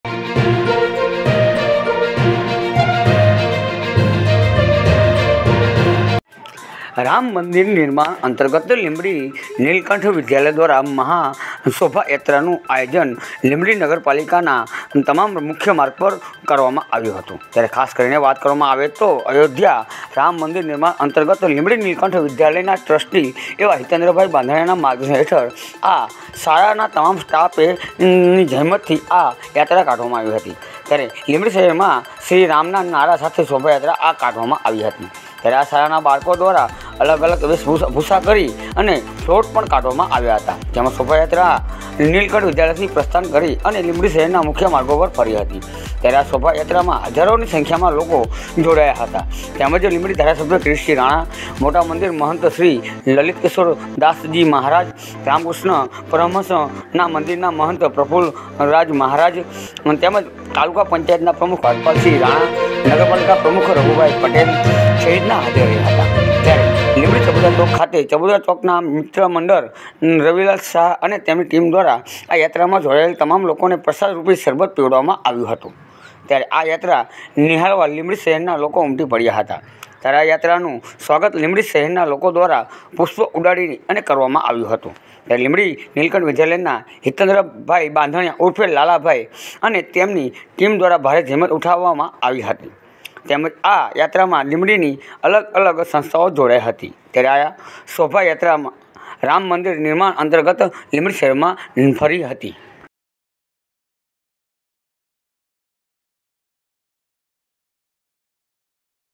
લીંબડીમા રામ મંદિર અર્તગત મહા શોભાયાત્રા નીકળી
રામ મંદિર નિર્માણ અર્તગત લીંબડી નીંલકંઠ વિદ્યાલય દ્વારા મહા યાત્રા લીંબડી શહેરના નગર પલીકાના મુખ્ય માર્ગો પર ફરી હતી

ત્યારે ખાસ કરીને વાત કરવામાં આવે તો અયોધ્યા રામ મંદિર નિર્માણ અર્તગત લીંબડી નીલકંઠ વિદ્યાલયના ટ્રસ્ટી હિતેન્દ્રભાઇ બાંધણીણાના માર્ગદર્શન હેઠળ અને આ શાળાના તમામ સ્ટાફની જહેમતથી લીંબડી શહેરમાં શ્રીરામના નારા સાથે શોભાયાત્રા કાઢવામાં આવી હતી ત્યારે આ શાળાના બાળકો દ્રારા અલગ અલગ વેશભુશાના ફ્લોટ કાઢવામાં આવ્યા હતા જેમાં આ શોભાયાત્રા નિલકંઠ વિધ્યાલયથી પ્રસ્તાન કરી અને લીંબડી શહેરના મુખ્ય માર્ગો પર ફરી હતી ત્યારે આ શોભા યાત્રામાં હજારોની સંખ્યામાં લોકો જોડાયા હતા તેમજ લીંબડી ધારાસભ્ય કિરીટસિંહ રાણા, મોટા મંદિર મહંત લલીતકિશોર મહારાજ , રામકૃષ્ણ મંદિરના મહંત પ્રફુલ મહારાજ, પંચાયત પ્રમુખ હરપાલસિહ રાણા, નગરપાલીકા પ્રમુખ રધુભાઇ પટેલ, સહિતના હાજર રહયા હતા ત્યારે લીંબડી ચબુતરા ચોક ખાતે ચબુતરા ચોક મિત્ર મંડળના રવિલાલ શાહ અને તેમની ટીમ દ્વારા આ યાત્રમાં જોડાયેલ તમામ લોકોને પ્રસાદરૂપી શરબત પીવડાવવામાં આવ્યું હતું ત્યારે આ યાત્ર નિહાળવા લીંબડી શહેરના લોકો ઉમટી પડયા હતા ત્યારે આ યાત્રનું સ્વાગ લીંબડી શહેરના લોકો દ્વારા પુષ્પ ઉડાડીને કરવામાં આવ્યું હતું ત્યારે લીંબડી નીલકંઠ વિદ્યાલયના હિતેન્દ્રભાઇ બાંધણીયા ઉર્ફે લાલાભાઇ અને તેમની ટીમ દ્વારા ભારે જહેમત ઉઠાવી હતી ત્યારે આ યાત્રમાં લીંબડીની અલગ અલગ સંસ્થાઓ પણ જોડાઇ હતી ત્યારે આ શોભાયાત્રા રામ મંદિર નિર્માણ અર્તગત લીંબડી શહેરમાં નિકળી હતી